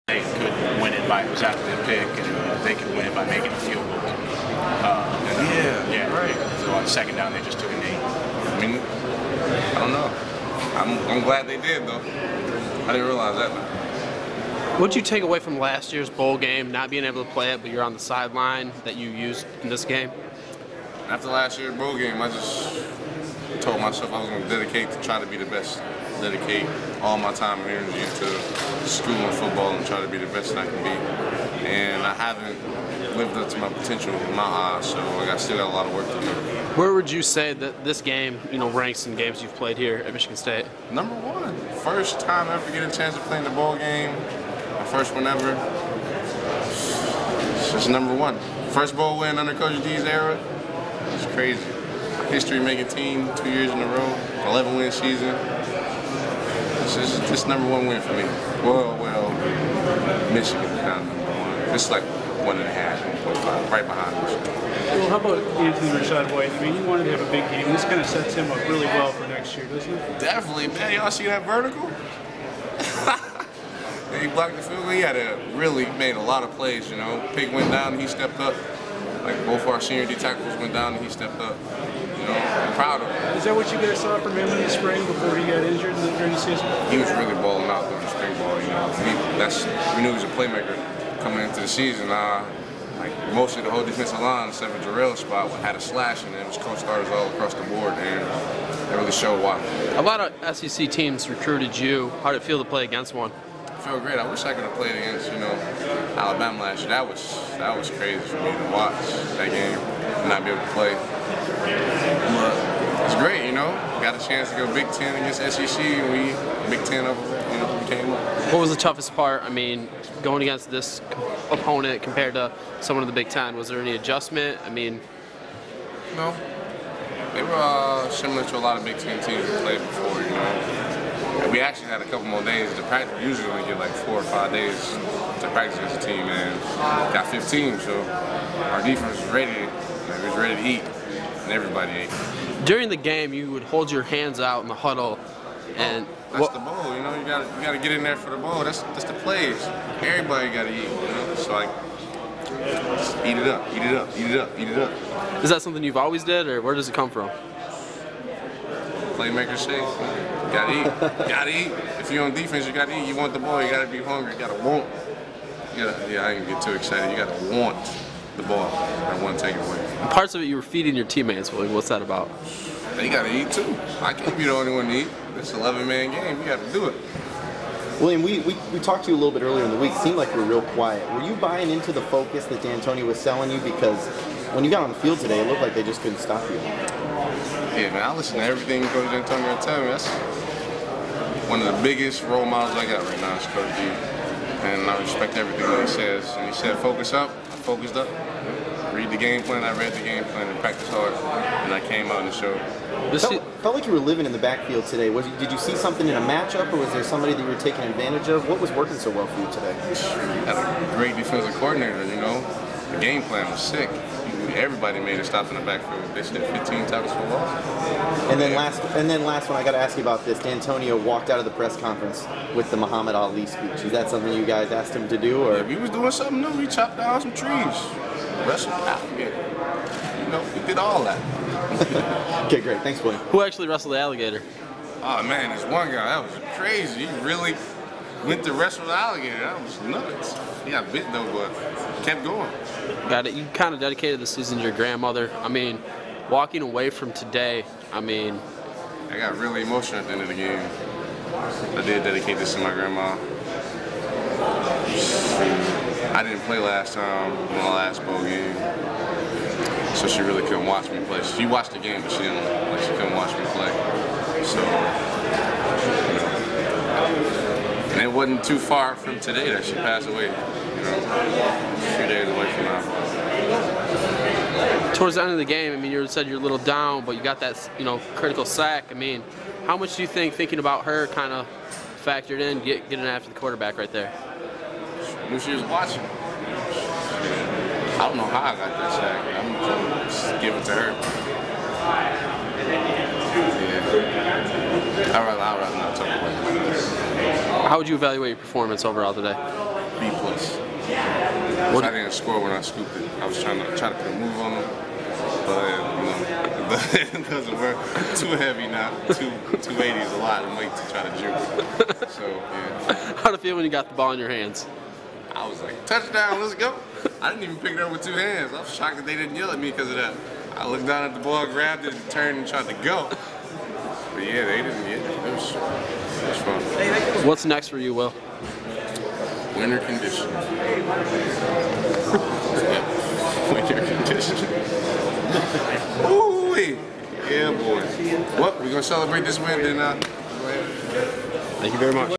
Will Gholston Outback Bowl Post Game (01/02/12)
William Gholston talks to the media after Michigan State's Outback Bowl win over Georgia. VIDEO BY SPARTAN TAILGATE VIDEOS.